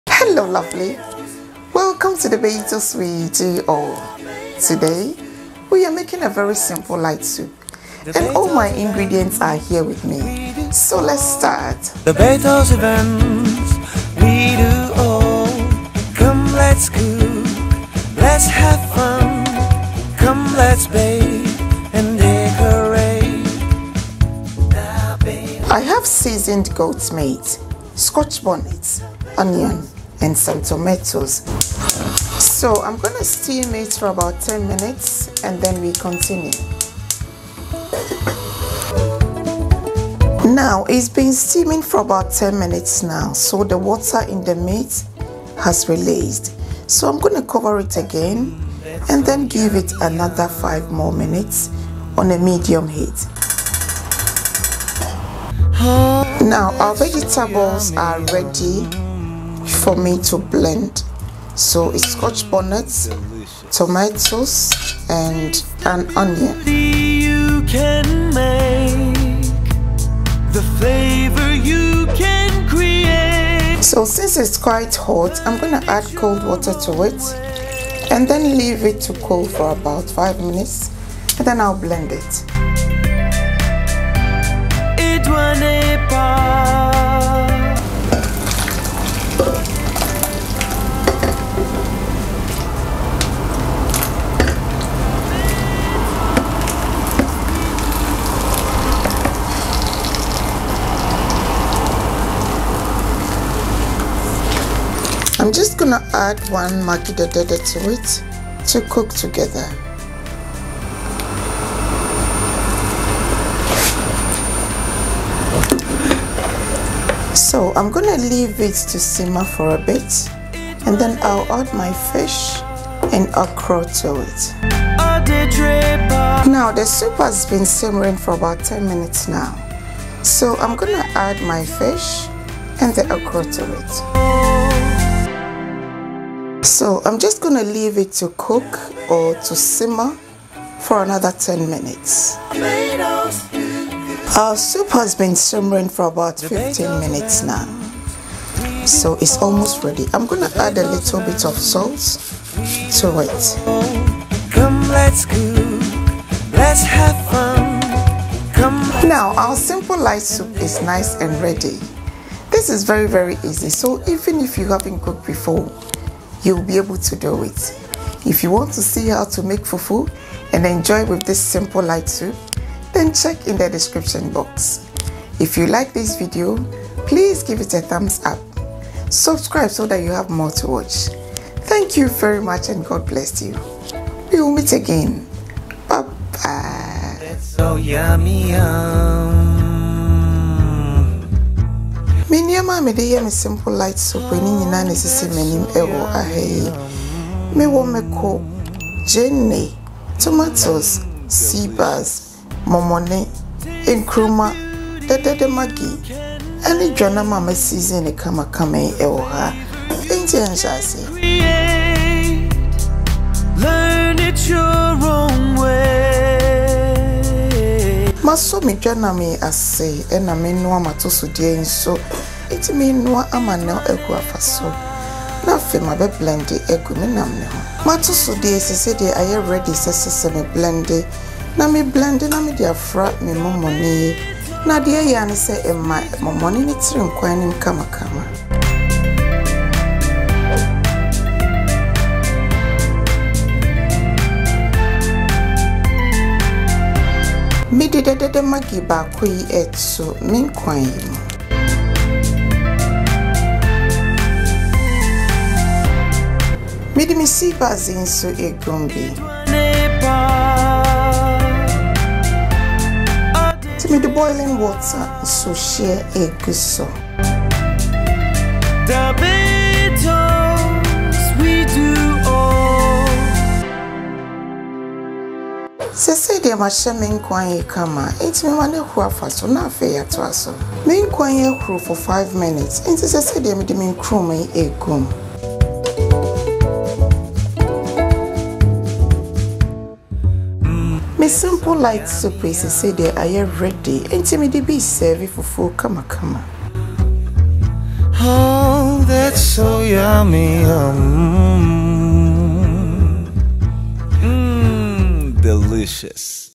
How to Make Simple Light Soup
Good evening lovely family! I trust your day was good!
Today, we made goat meat and fish soup!

Ingredients include:
- Seasoned Goat Meat (seasoned with Remie Goat Seasoning, Natural Spices)
- Sea Bass Fish
- Scotch Bonnet
- Onion
- Tomatoes
- Maggi D3d33d3

This recipe is very easy to prepare after the day's work and everyone can enjoy!